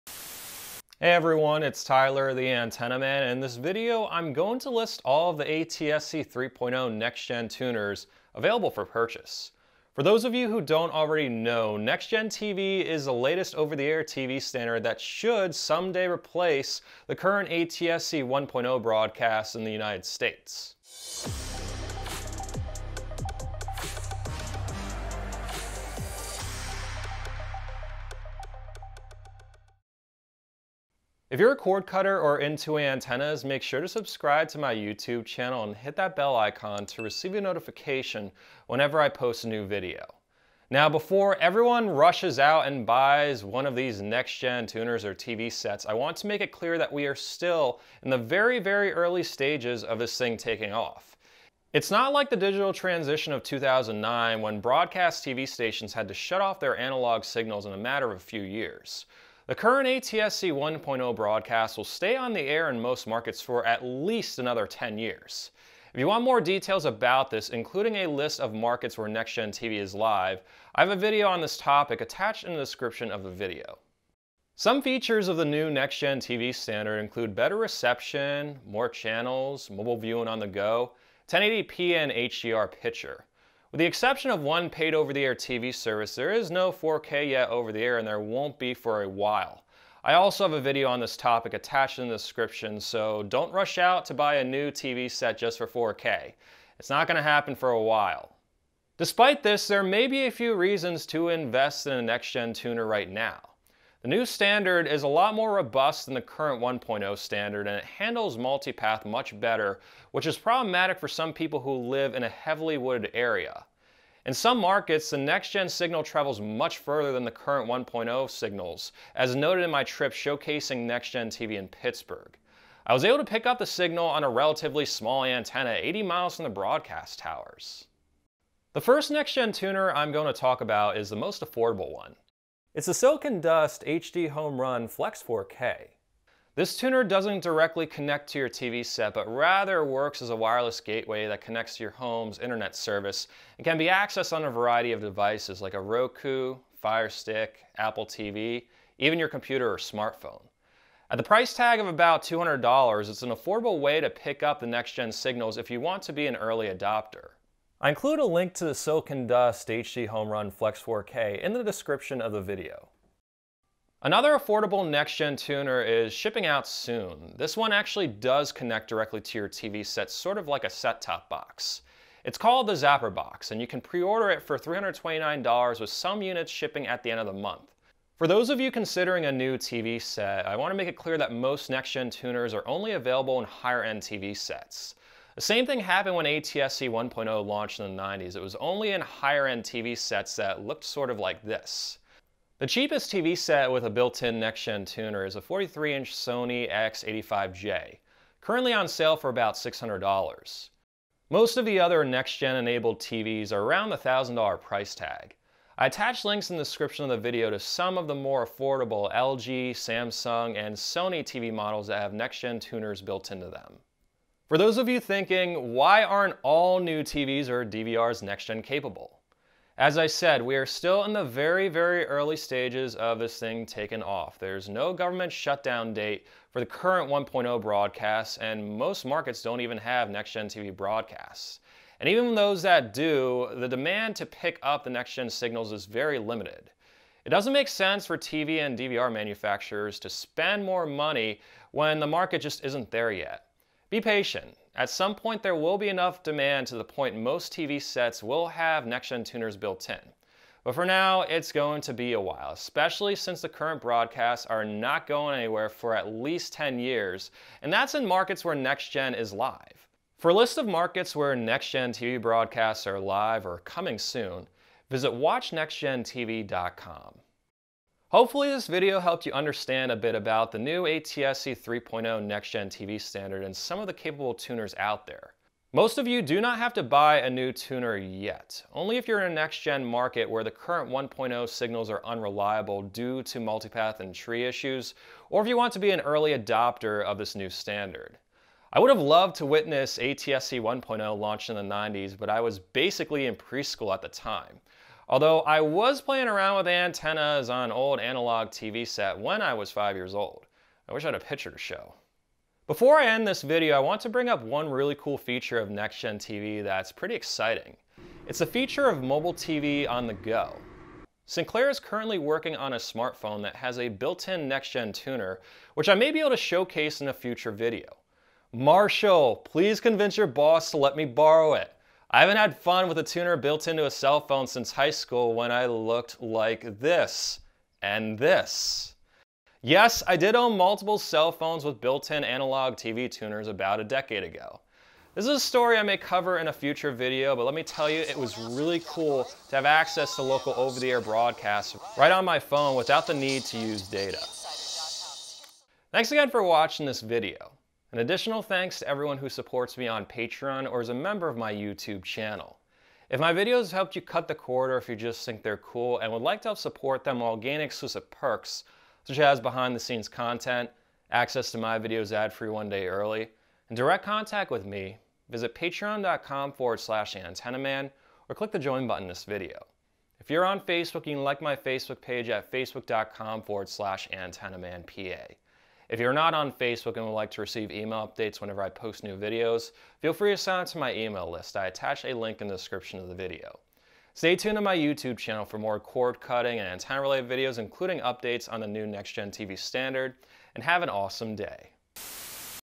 All ATSC 3.0 NextGen TV Sets and Tuners Available for Purchase
In this video I discuss ATSC 3.0 NextGen TV along with the TV sets and tuners that are currently available for purchase. Features of the new NextGen TV standard include better reception, more channels, 1080p/HDR picture (no 4K yet) and mobile viewing on the go. If your market has live NextGen broadcasts - should you invest in a new tuner?

Link to the Silicon Dust HDHomerun Flex 4K Tuner: